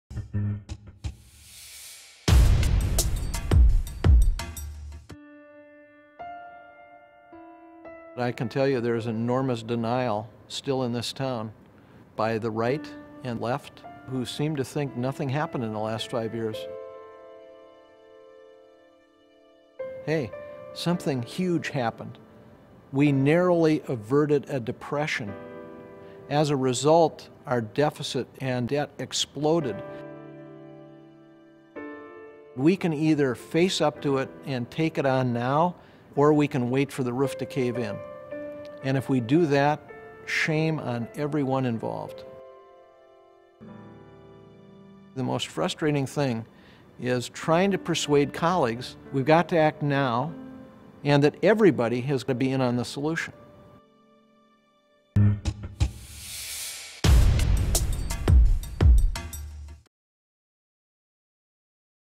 Overdraft "100 Words" -- Debt Denial -- Kent Conrad
Senate Budget Chairman Kent Conrad shares his concerns about the urgency of the debt crisis.